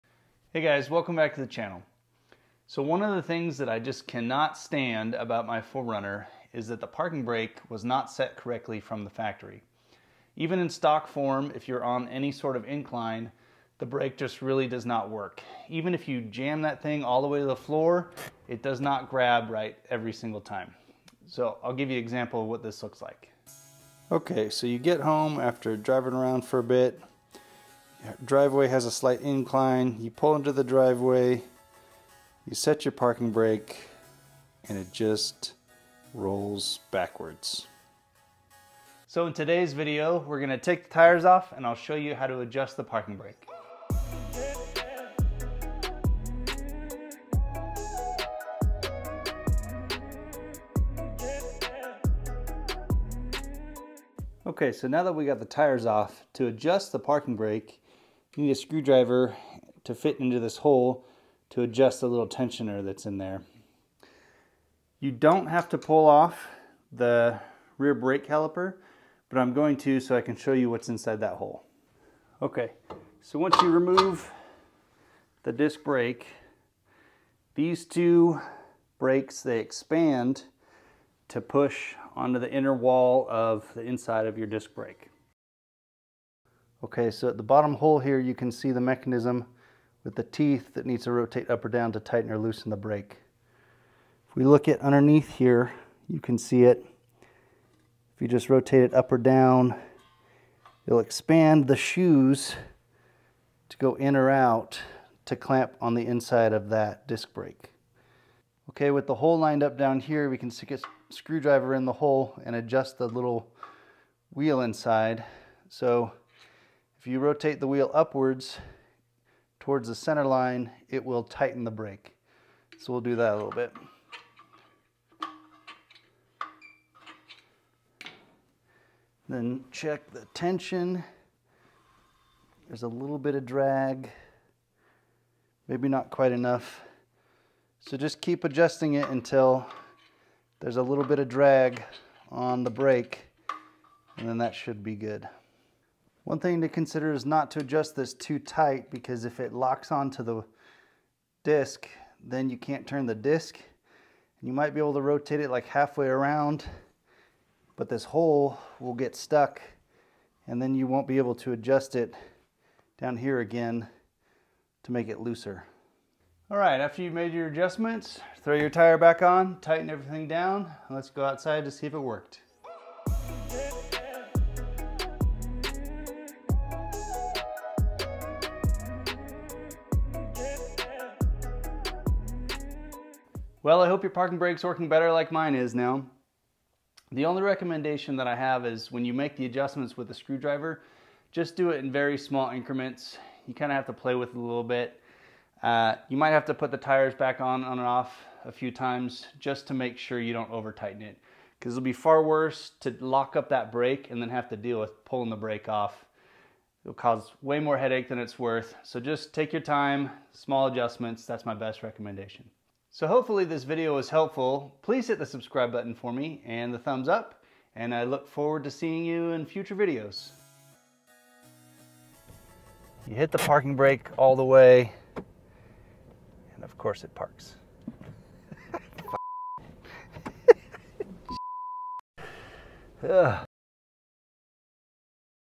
5th Gen Toyota 4Runner Parking Brake Adjustment
My Parking Brake never worked correctly on my 2021 4Runner.  I'll show you how to fix that if you have the same problem!

Timecode:
0:00 - Intro
0:24 - Parking Brake Not Working On Incline
0:38 - Taking The Tires Off
0:54 - How To Make Adjustments
1:26 - What The Adjustors Look Like
2:46 - Did It Work?
3:03 - Conclusion
3:40 - Outtake